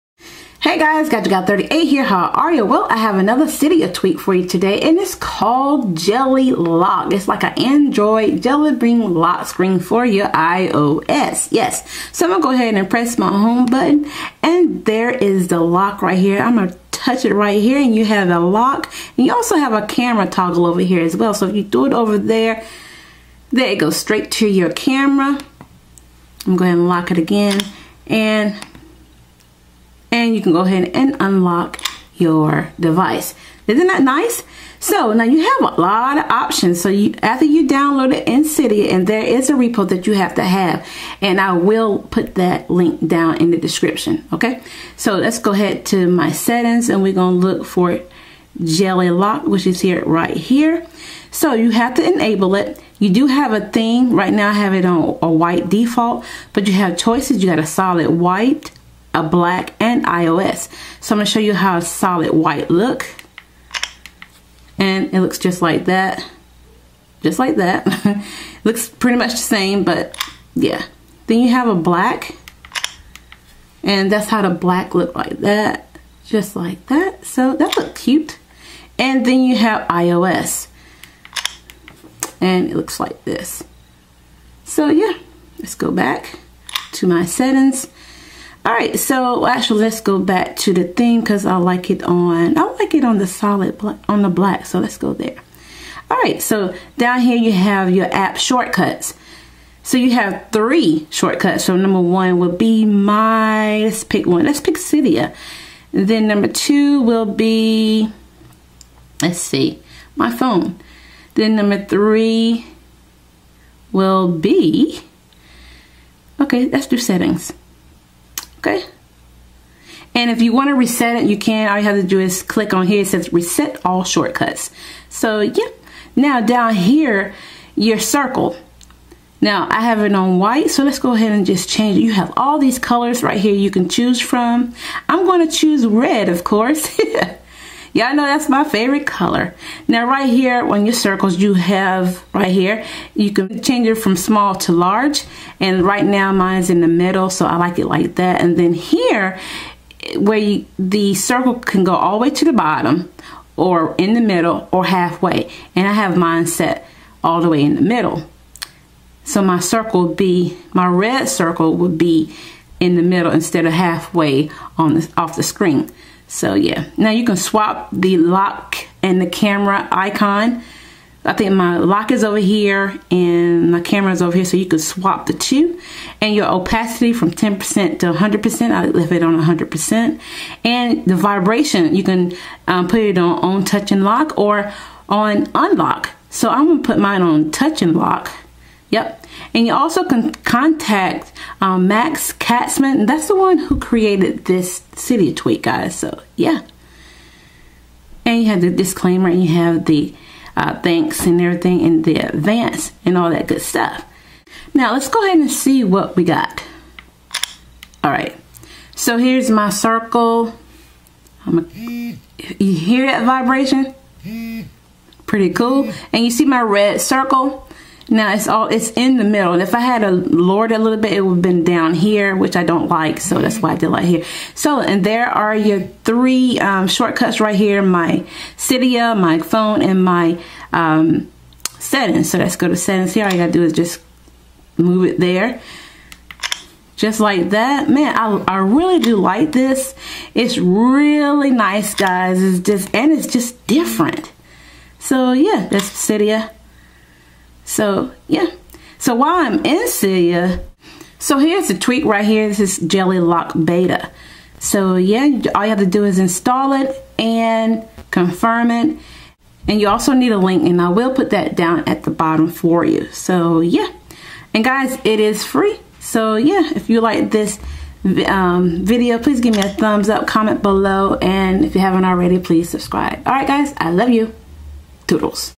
JELLYLOCK Cydia Tweak
In this video I show you a cydia tweak call JellyLock for your lockscreen.

Price: FREE
Author: Max Katzmann